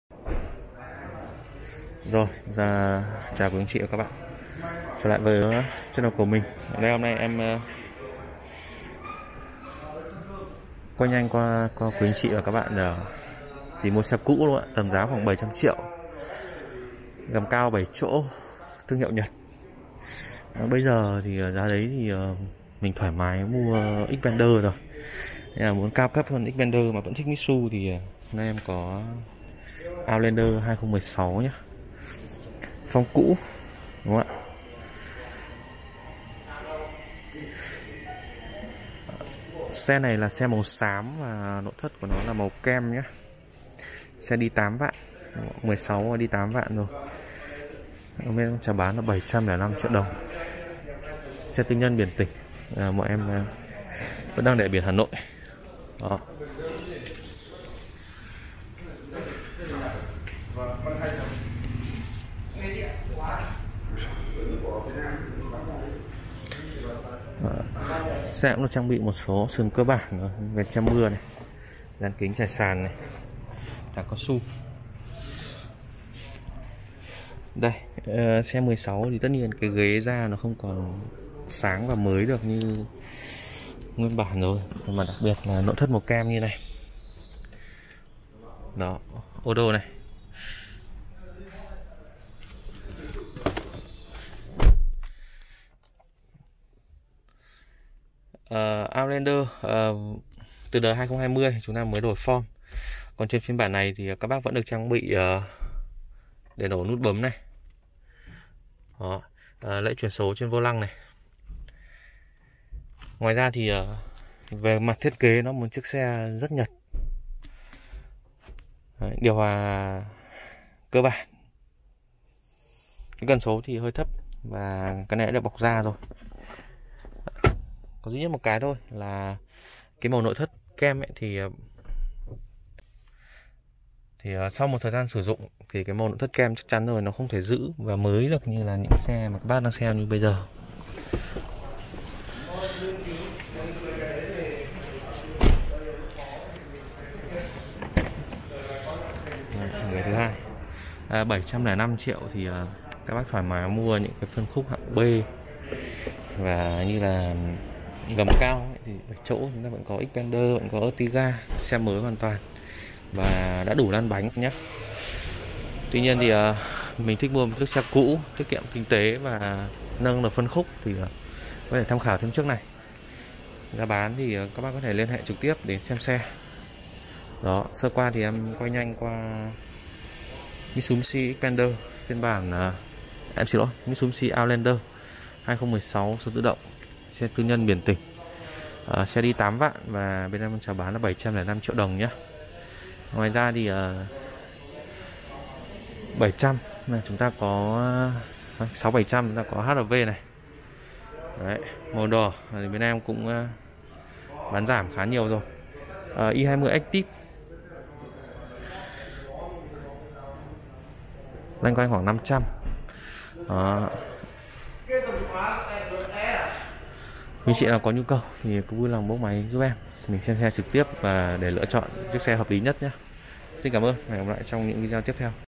Bán nhanh Mitsubishi Outlander 2016 phom cũ | lựa chọn xe cũ 7 chỗ cho gia đình giá vừa phải
+ Hotline Tư Vấn     : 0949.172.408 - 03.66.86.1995